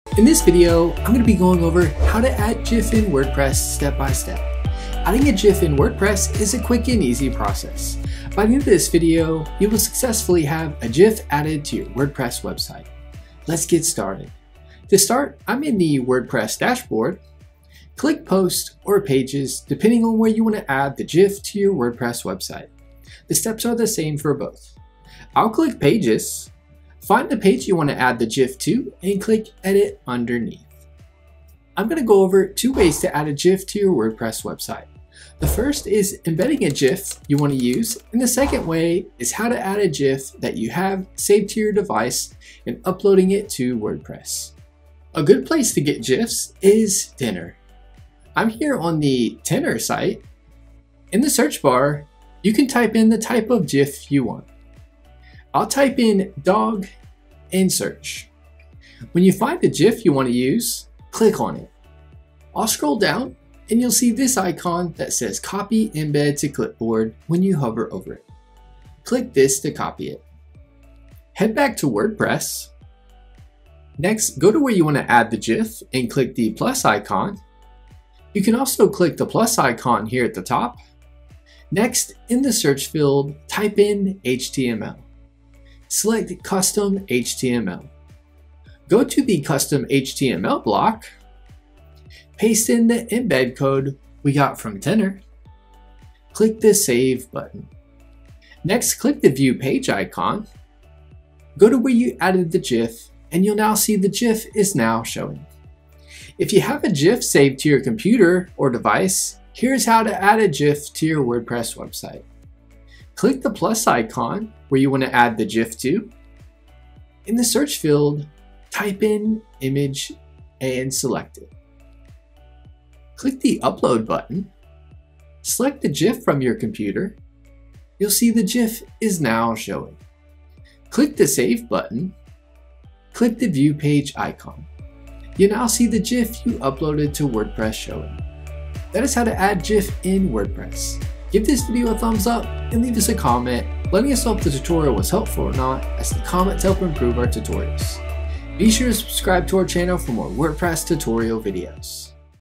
How To Add GIF In WordPress 2026 🔥 - (FAST & Easy!)
Learn how to add gifs in a WordPress website step by step. Adding gifs in WordPress is a quick and easy process.

Let's get started. To start, I’m in the WordPress dashboard. Click Posts or Pages depending on where you want to add the gif to for your WordPress website. The steps are the same for both. I’ll click Pages. Find the page you want to add the gif to and click edit underneath.

I’m going to go over two ways to add a gif to your WordPress website. The first, is embedding a gif you want to use and the second way is how to add a gif that you have saved to your device and uploading it to WordPress.

A good place to get gifs is Tenor. I’m here on the Tenor site. In the search bar, you can type in the type of gif you want. I’ll type in dog and search. When you find a gif you want to use click on it. I’ll scroll down and you’ll see this icon that says copy embed to clipboard when you hover over it. Click this to copy it.

Head back to WordPress. Next, go to where you want to add the gif and click the plus icon. You can also click the plus icon here at the top. Next, in the search field type in HTML. Select Custom HTML.

Go to the Custom HTML block. Paste in the embed code we copied from Giphy. Click the Save button. Next, click the View Page icon. Go to where you added the Gif and you’ll now see the Gif is now showing.

If you have a gif saved to your computer or device here is how to add a gif to your WordPress website.

Click the plus icon where you want to add the gif to. In the search field, type in image and select it. Click the Upload button. Select the gif from your computer. You’ll see the gif is now showing. Click the save button. Click the view page icon. You’ll now see the gif you uploaded to WordPress showing.

That is how to add gif in WordPress.

Help With WordPress:
Fiverr: